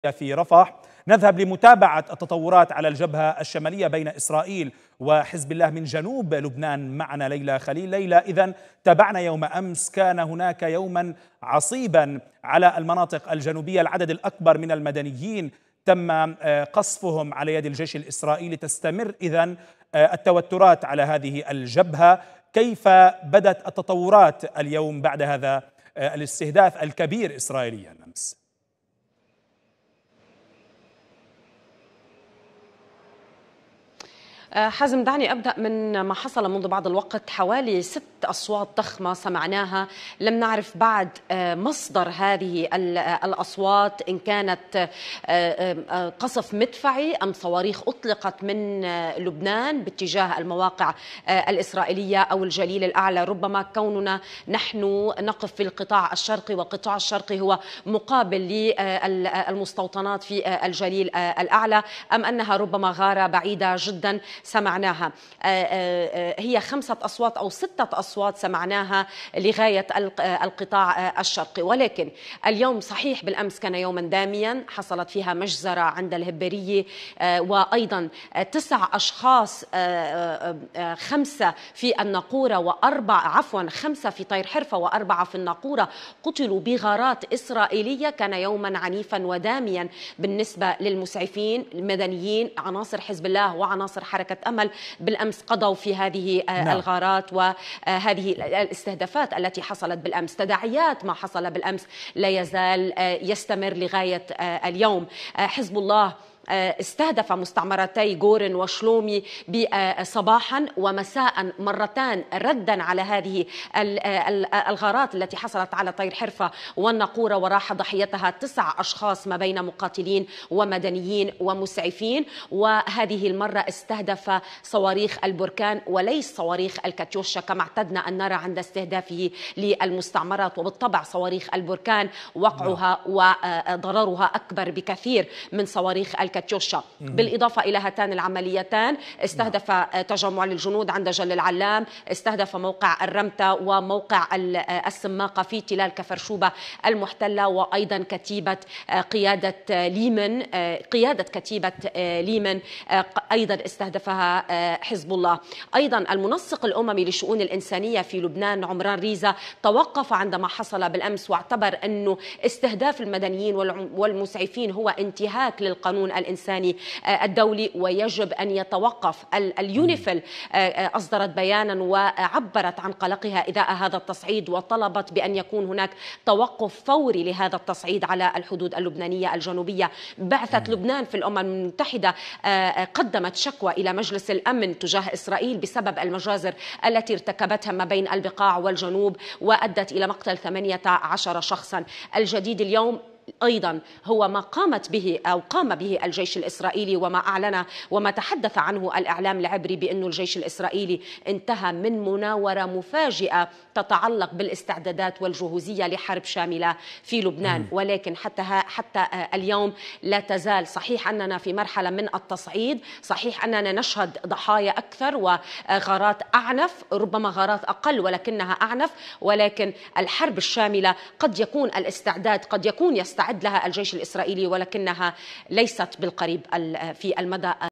وقوع 6 انفجارات غير معلومة المصدر على الحدود اللبنانية مع إسرائيل.. مراسلتنا تنقل الصورة
وقوع 6 انفجارات غير معلومة المصدر على الحدود اللبنانية مع إسرائيل.. مراسلتنا تنقل الصورة.. التفاصيل في تقرير مراسلتنا ليلى خليل

مزيد من التفاصيل مع مراسلة الغد من جنوب لبنان ليلى خليل مع الإعلامية سينار سعيد.